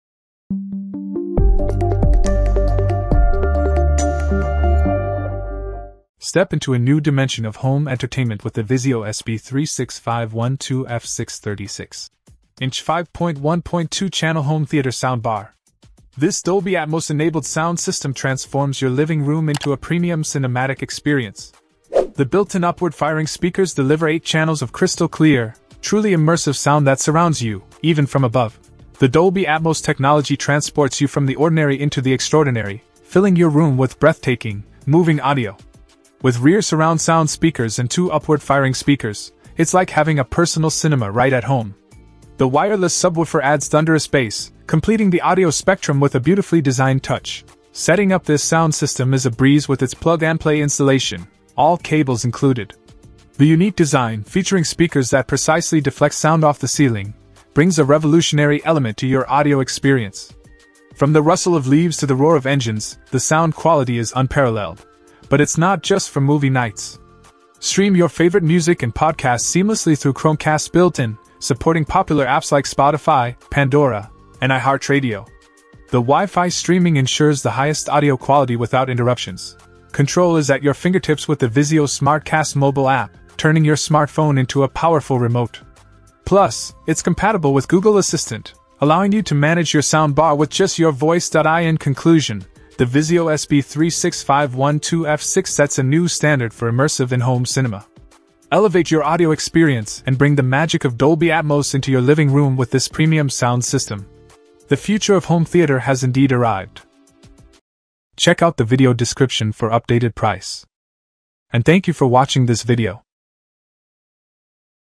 Immersive Cinematic Audio: VIZIO SB36512-F6 5.1.2 Dolby Atmos Sound Bar Review
0:00 Intro
0:06 Review

Things that we mentioned in this video:
VIZIO SB36512-F6 36” 5.1.2 : B07JDMBK2N